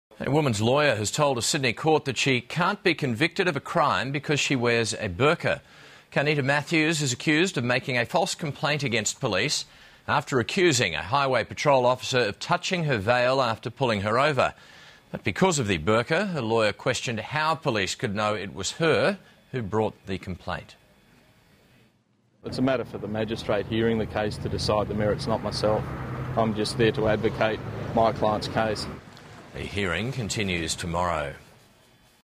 Devout muslim woman accuses police of racism
A policeman accused of trying to pull off a Muslim woman's headdress has told a NSW court he isn't a racist.